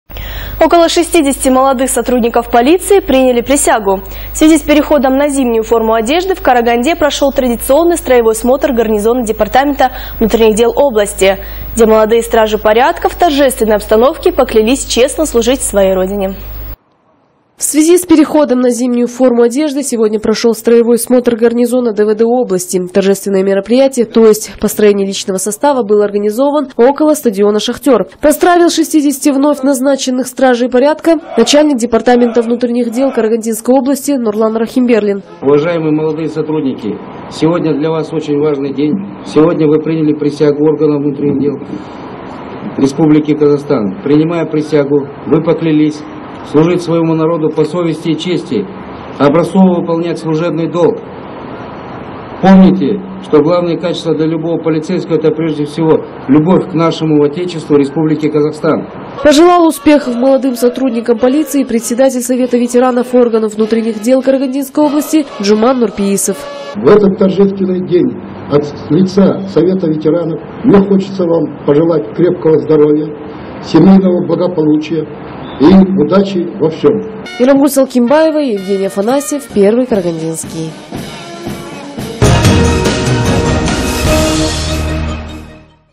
30_10_12_присяга
Около 60 молодых сотрудников полиции приняли присягу. В связи с переходом на зимнюю форму одежды в Караганде прошел традиционный строевой смотр гарнизона департамента внутренних дел области. Где молодые стражи порядка в торжественной обстановке поклялись честно служить своей Родине